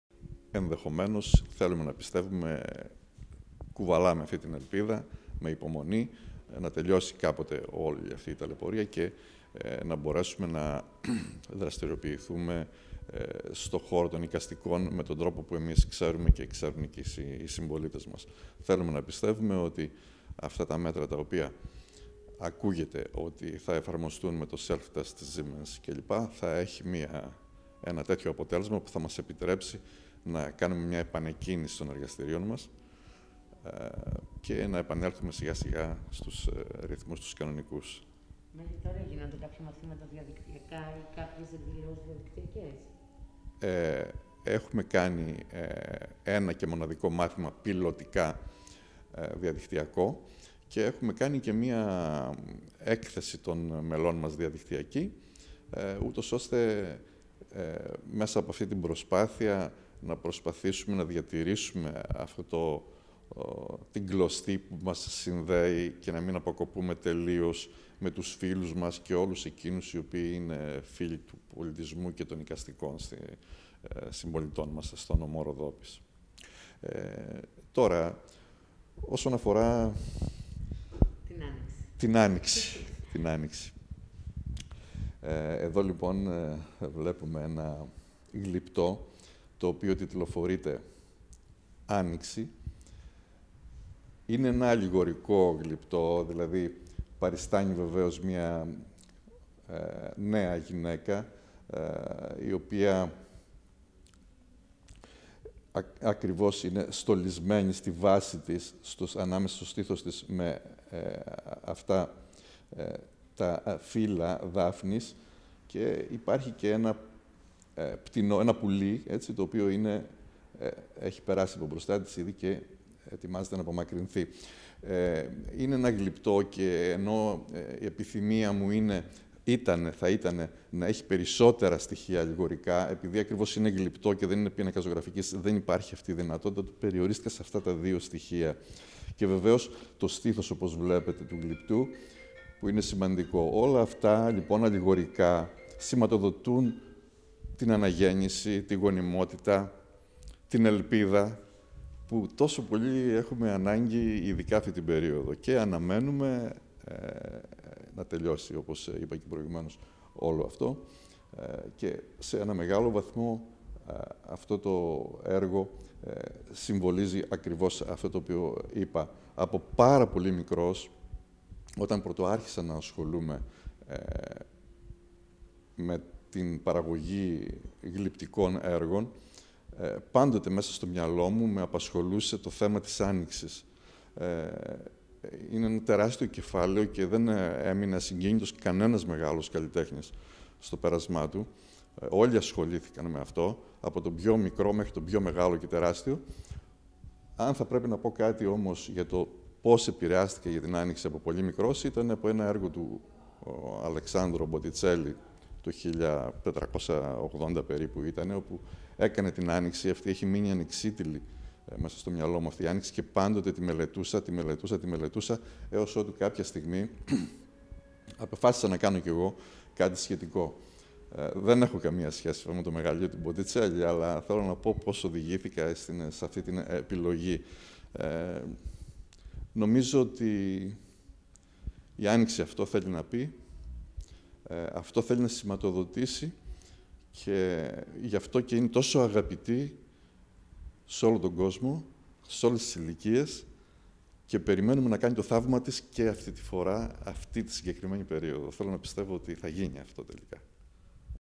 thrakionline.gr:ΤΗΝ "ΑΝΟΙΞΗ" ΜΑΣ ΠΑΡΟΥΣΙΑΣΕ Ο ΑΡΗΣ ΑΣΣΑΡΙΩΤΑΚΗΣ
thrakionline.gr:Το γλυπτό του που τιτλοφορείται "άνοιξη" μας παρουσίασε ο πρόεδρος του συλλόγου καλλιτεχνών Ροδόπης "ο ΑΘΗΝΙΩΝ" Άρης Ασσαριωτακης , ας δούμε τι είπε για την έμπνευση που είχε και για το μήνυμα που θέλει να περάσει...